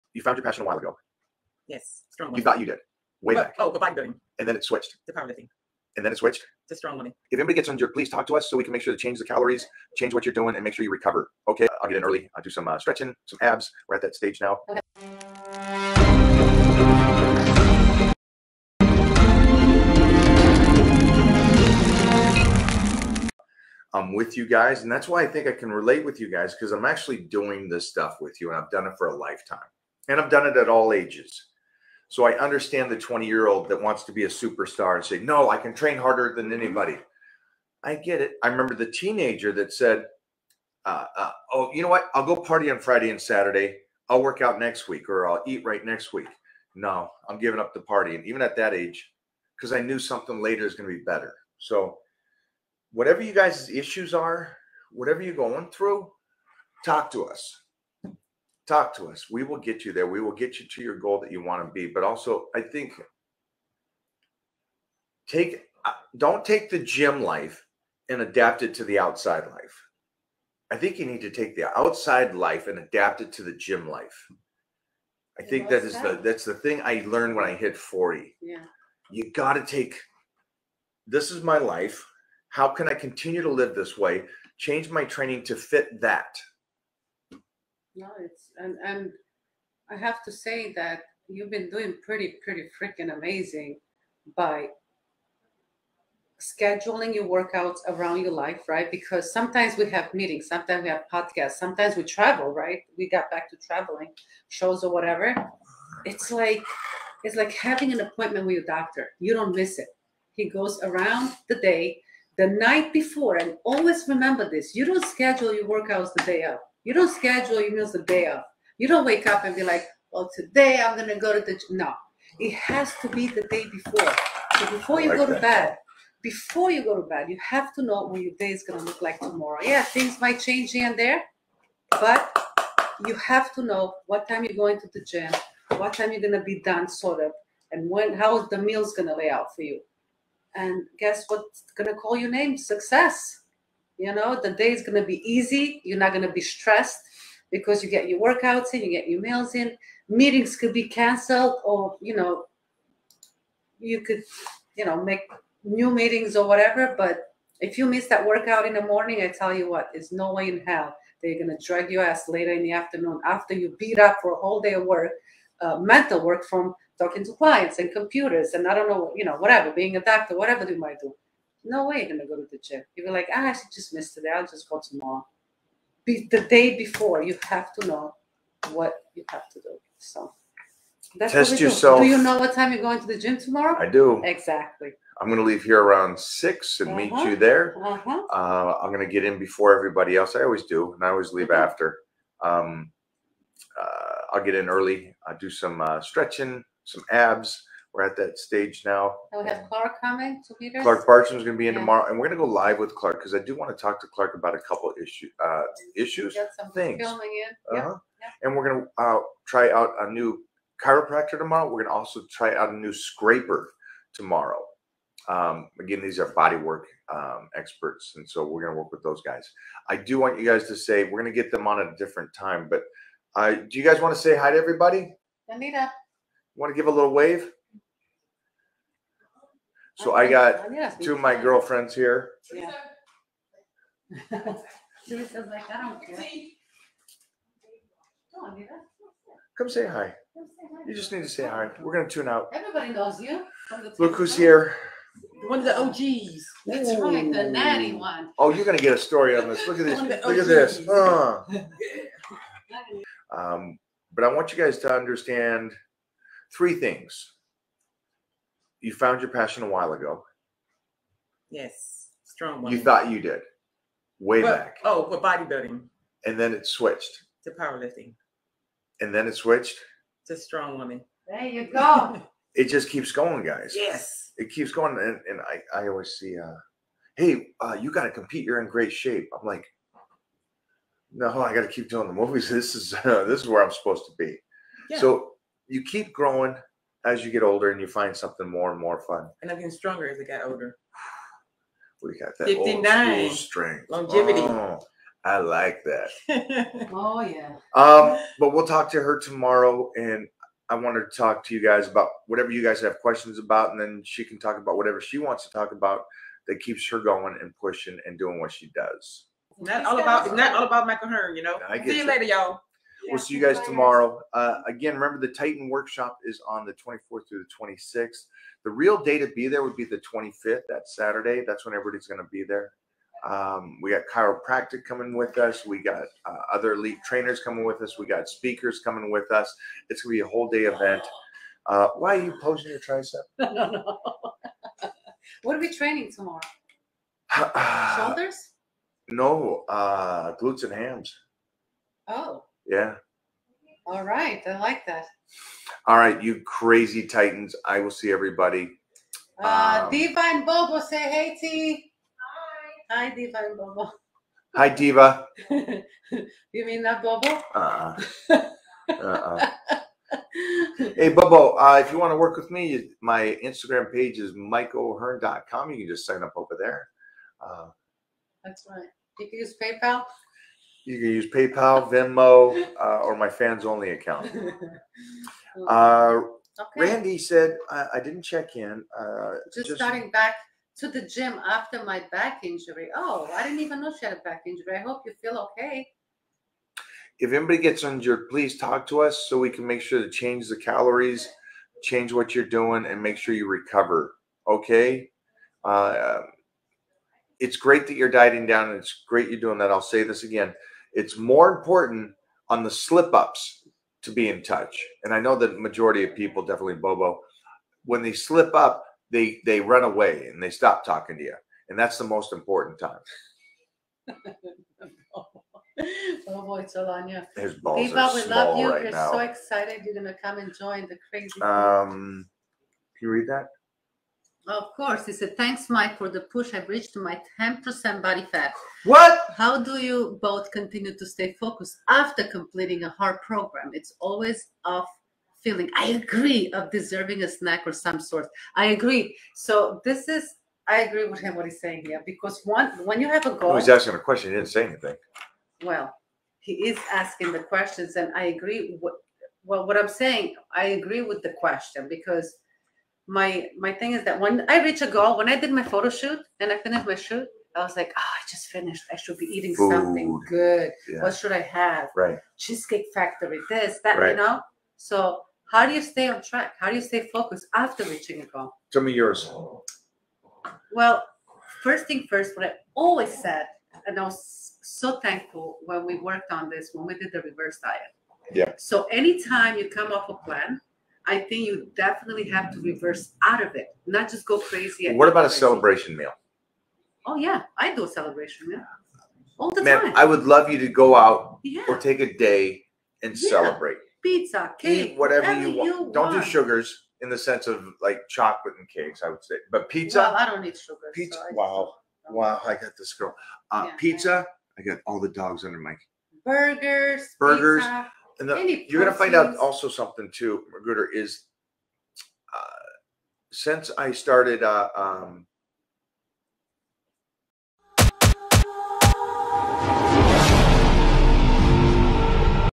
Mike O'Hearn -Powerlifting Vs Bodybuilidng
Hi this is Mike O'Hearn . In this video you will see
 mike talking about powerlifting and bodybuilding.

Watch the video till end and if you like the video don't forget to

thanks